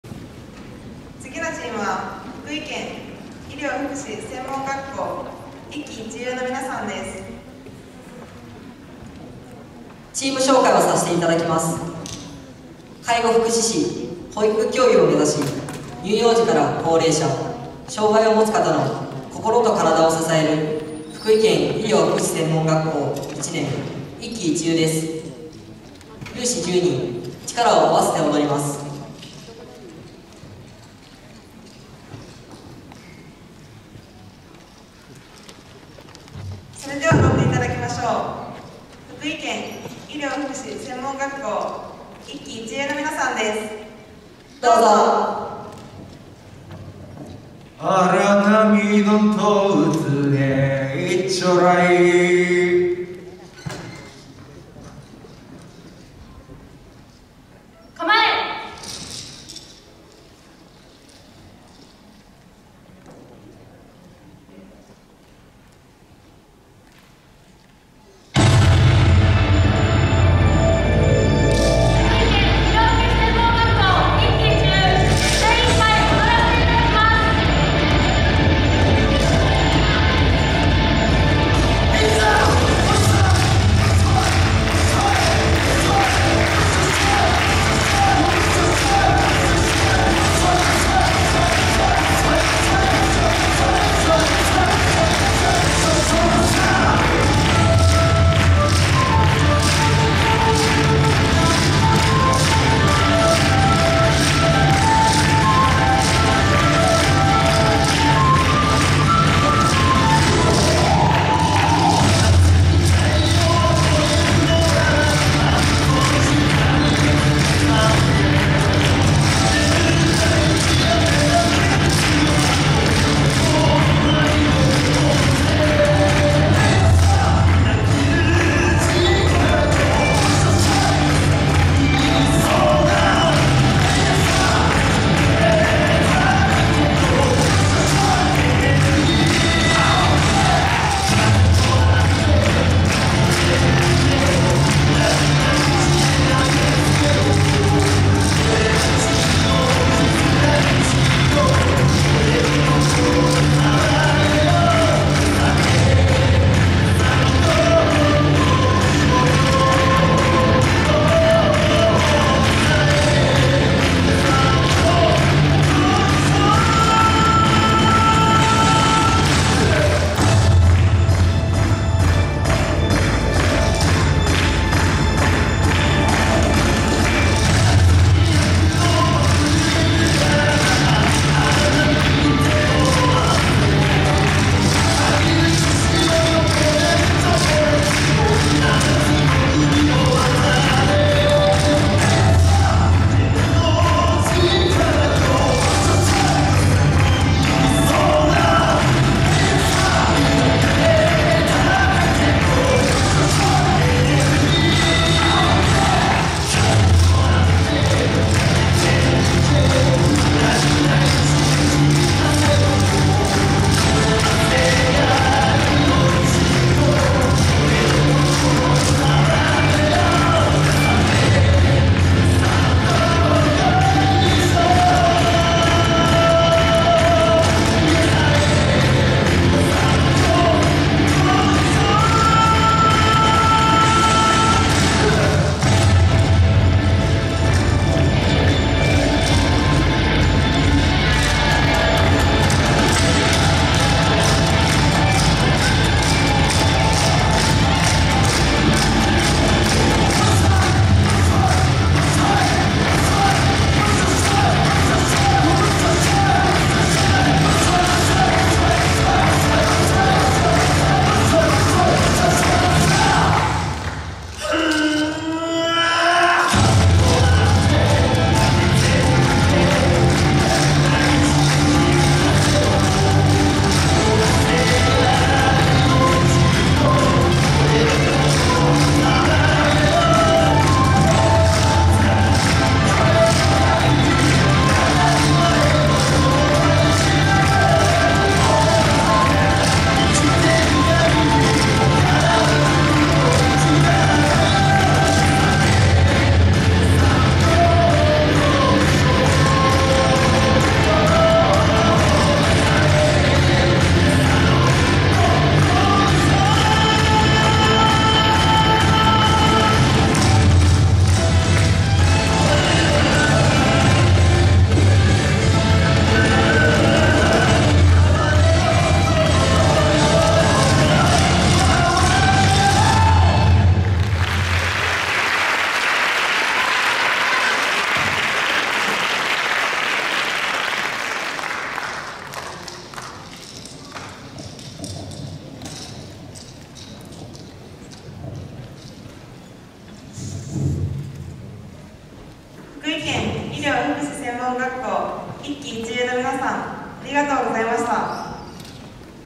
福井県医療福祉専門学校～一軌一悠～ 1回目
2021年10月23日「福井つぐみ祭り」